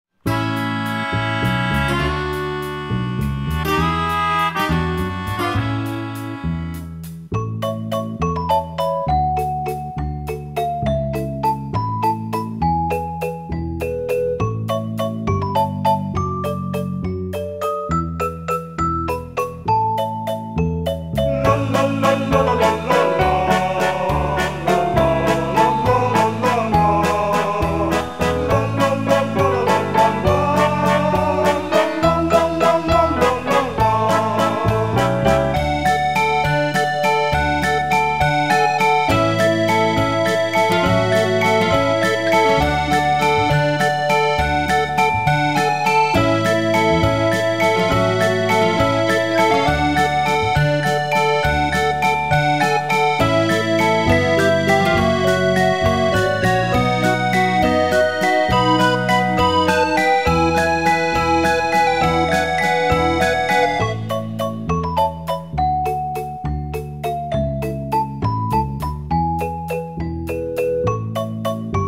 Masha e Orso - Sfilata Carro di Carnevale Ispirato al Film per Bambini
A Guidonia, la sfilata del carro di Carnevale di Masha e Orso, ispirato al film per bambini